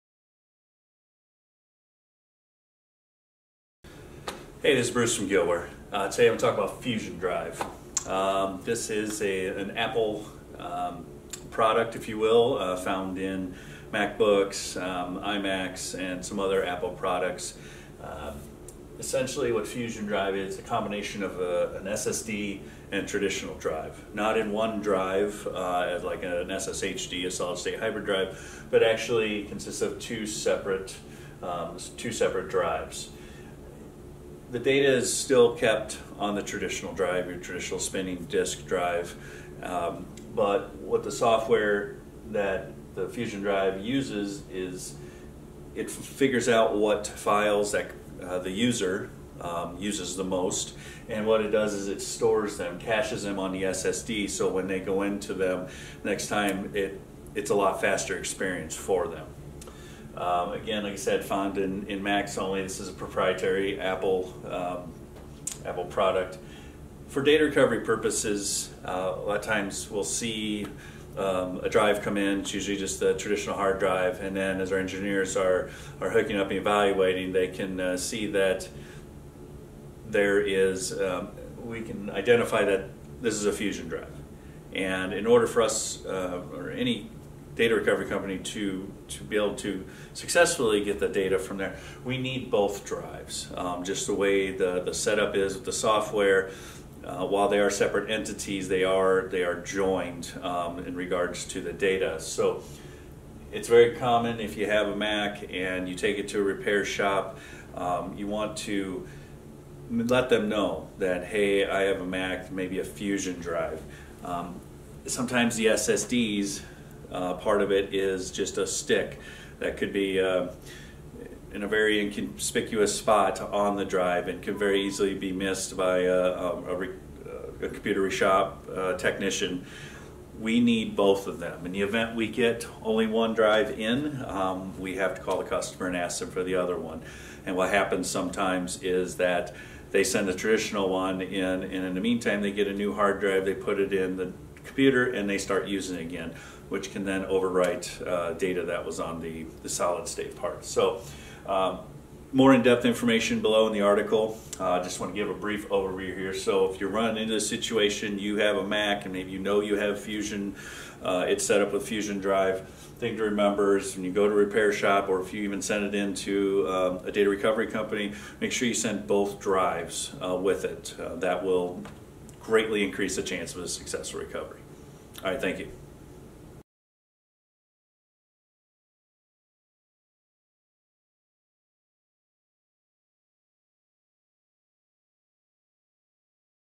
Recover data from a Fusion Drive
Apple introduced its Fusion Drive technology in 2012 to marry speedy solid state flash memory with traditional hard disk drives. The Fusion Drive is a proprietary system developed by Apple. It is available for the iMac and Mac Mini; however, modern Macbooks and Mac Pros exclusively use flash memory. Fusion Drives work entirely under-the-hood, and some users (and computer repair professionals) might not even know if the Mac they’re working on has a Fusion Drive.

This can have unexpected consequences when it comes to Fusion Drive data recovery. The SSD used in Fusion Drive setups is a small, tucked-away M.2 solid state drive that can easily go unnoticed by both the user of a disobedient Mac, and the computer repair technician who assesses the computer and decides its hard drive needs to go to a data recovery lab.

Due to the way Fusion Drives work, we need both the traditional hard drive and the solid state drive in order to recover the user’s data. Sometimes, we’ll receive the hard drive from a Mac that, unbeknownst to the client, belonged to a Fusion Drive setup. While we work on the drive, the user may have already replaced the old hard drive and started using their Mac again. This damages the data on the SSD and makes our job much, much more difficult—not to mention, it also makes our chances of successfully recovering our client’s data much lower.